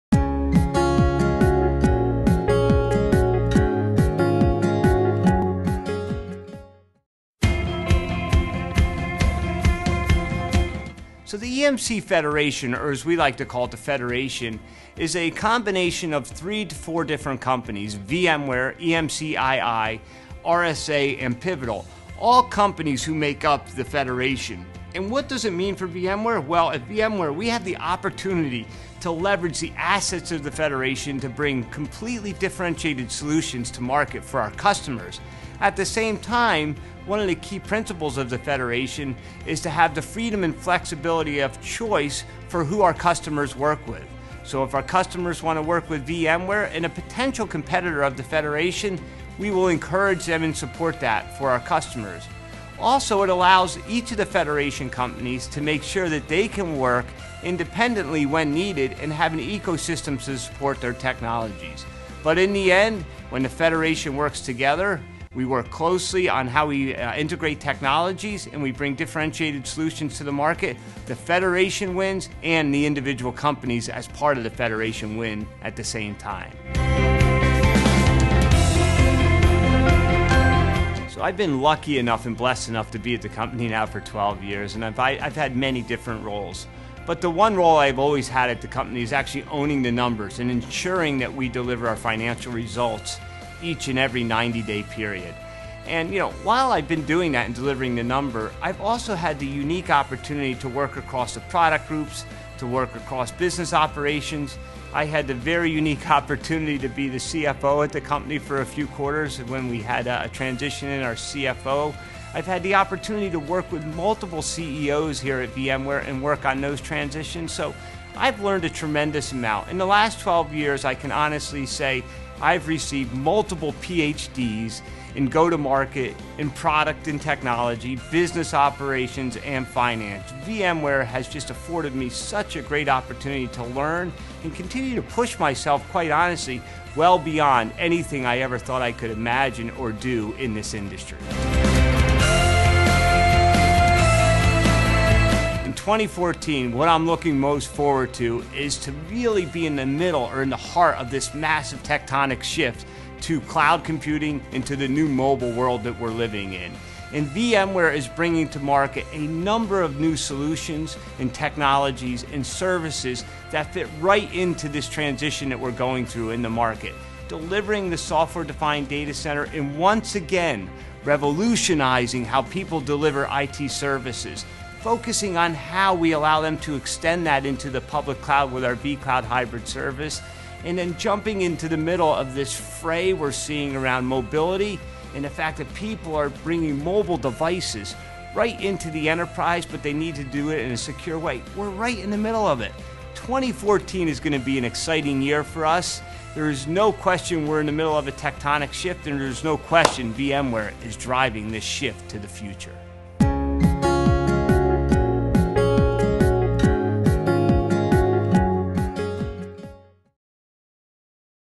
Leadership Perspectives: Carl Eschenbach, Part 2
In Part Two of a two-part series, Carl Eschenbach, President and Chief Operating Officer talks about The EMC Federation, what he's learned from his role at VMware and what he's looking forward to in 2014. More Leadership Perspectives content and videos: